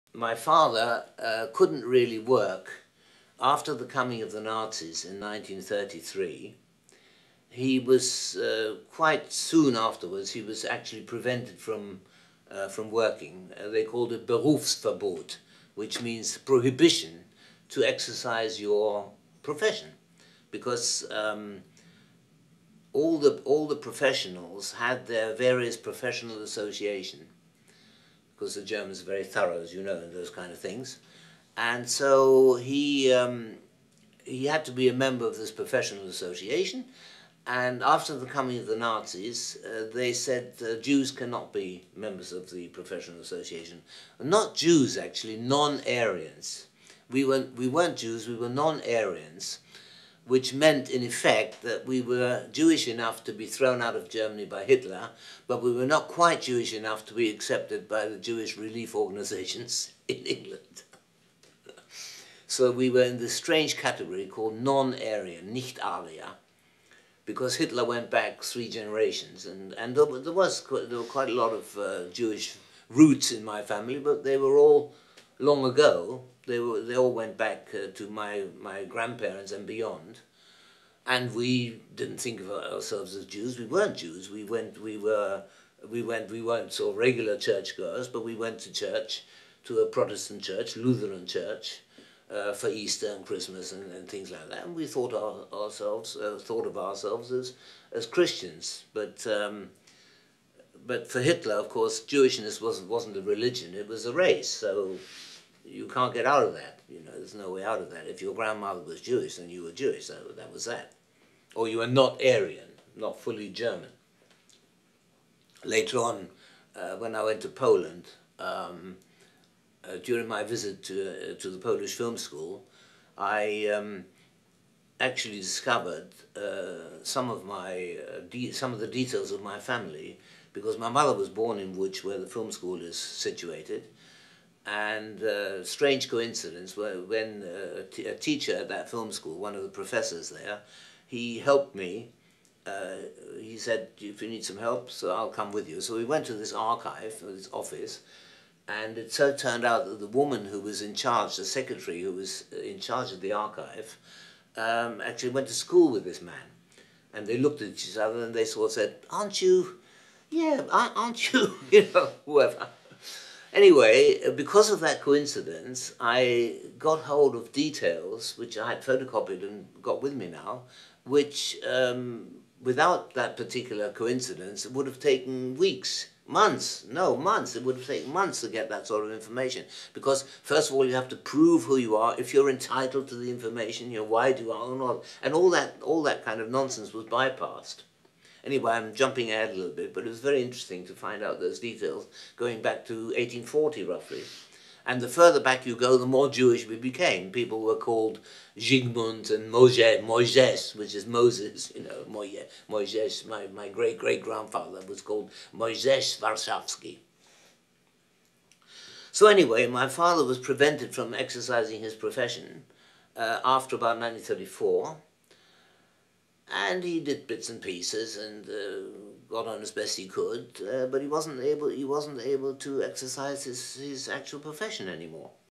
Walter Lassally - Jewish, but not Jewish enough (2/266)
Born in Germany, cinematographer Walter Lassally (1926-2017) was best known for his Oscar-winning work on 'Zorba the Greek'. He was associated with the Free Cinema movement in the 1950s, and the British New Wave in the early 1960s. [Listener: Peter Bowen; date recorded: 2004]

TRANSCRIPT: My father couldn't really work after the coming of the Nazis in 1933. Quite soon afterwards, he was actually prevented from working. They called it 'Berufsverbot' which means prohibition to exercise your profession, because all the professionals had their various professional association, because the Germans are very thorough, as you know, in those kinds of things. So, he had to be a member of this professional association and after the coming of the Nazis they said Jews cannot be members of the professional association. Not Jews, actually, non-Aryans. We weren't Jews we were non-Aryans which meant, in effect, that we were Jewish enough to be thrown out of Germany by Hitler, but we were not quite Jewish enough to be accepted by the Jewish Relief Organisations in England. So we were in this strange category called non-Aryan, 'Nicht Arier', because Hitler went back three generations, and there were quite a lot of Jewish roots in my family, but they were all long ago. They all went back to my grandparents and beyond, and we didn't think of ourselves as Jews. We weren't Jews, we weren't, sort of, regular churchgoers, but we went to church, to a Protestant church, Lutheran church, for Easter and Christmas and things like that. We thought of ourselves as Christians, but for Hitler, of course, Jewishness wasn't a religion, it was a race, so you can't get out of that, you know. There's no way out of that. If your grandmother was Jewish, then you were Jewish, that was that; or you were not Aryan, not fully German.

Later on when I went to Poland, during my visit to the Polish Film School, I actually discovered some of the details of my family, because my mother was born in Łódź where the film school is situated. And, strange coincidence, a teacher at that film school, one of the professors there, he helped me. He said, if you need some help, I'll come with you. So we went to this archive, this office, and it so turned out that the woman who was in charge, the secretary who was in charge of the archive, actually went to school this man. And they looked at each other and they sort of said: aren't you? Yeah, aren't you? You know, whoever. Anyway, because of that coincidence I got hold of details which I photocopied and got with me now, which, without that particular coincidence, it would've taken weeks, no months, it would've taken months to get that sort of information. Because, first of all, you have to prove who you are, if you're entitled to the information, why, and all that kind of nonsense, was bypassed. Anyway, I'm jumping ahead a little bit, but it was very interesting to find out those details, going back to 1840, roughly. The further back you go, the more Jewish we became. People were called Zsigmund and Mojżesz, which is Moses, you know, Mojżesz. My great, great grandfather was called Mojzesz Warszawski.

So anyway, my father was prevented from exercising his profession after about 1934 and he did bits and pieces and got on as best he could, but he wasn't able to exercise his actual profession any more.